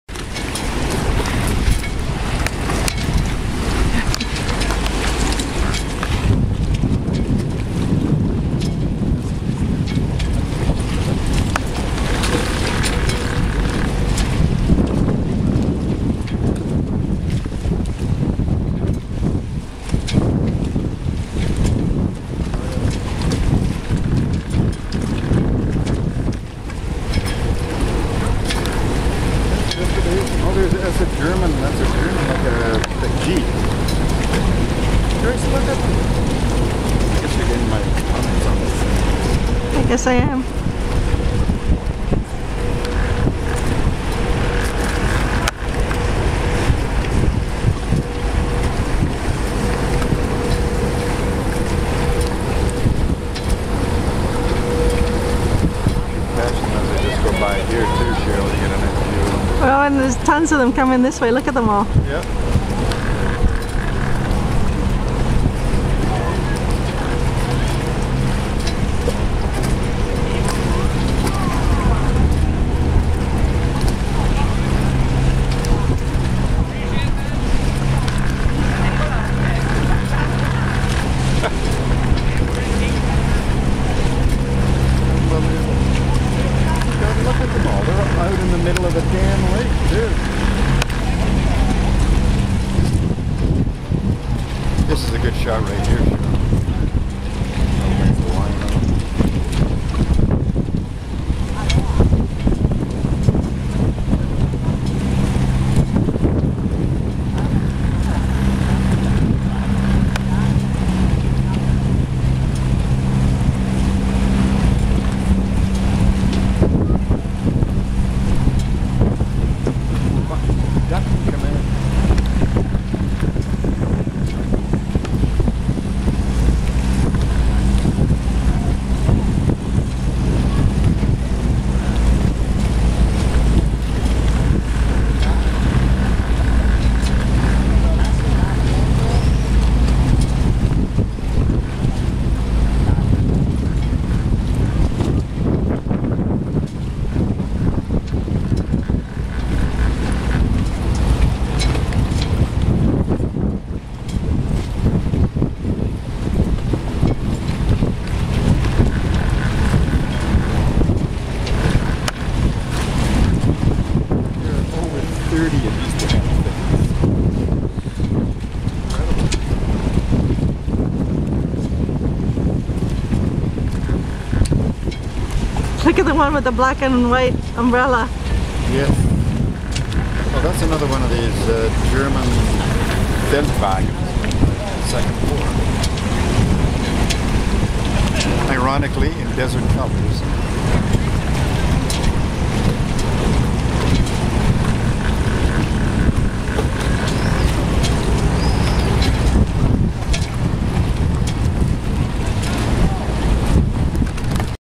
Amphibious vehicle parade - Lago di Garda - Salò 23 June 2013
Most unusual boat parade ever!  Amphibian cars!